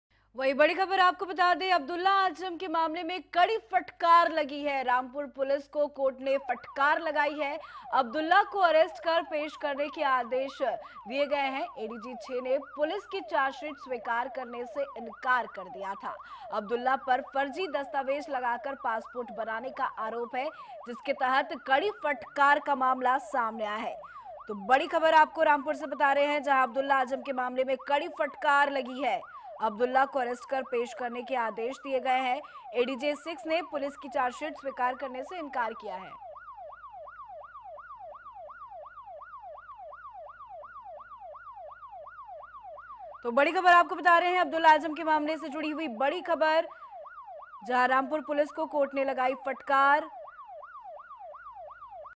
Abdullah Azam Khan का मामला, कोर्ट ने रामपुर पुलिस पर लगाई कड़ी फटकार R9TV
अब्दुल्ला आजम के मामले में कोर्ट ने रामपुर पुलिस पर कड़ी फटकार लगाई...अब्दुल्ला को अरेस्ट कर पेश करने के आदेश दिए है... ADJ-6 ने पुलिस की चार्जशीट स्वीकार करने से इनकार किया...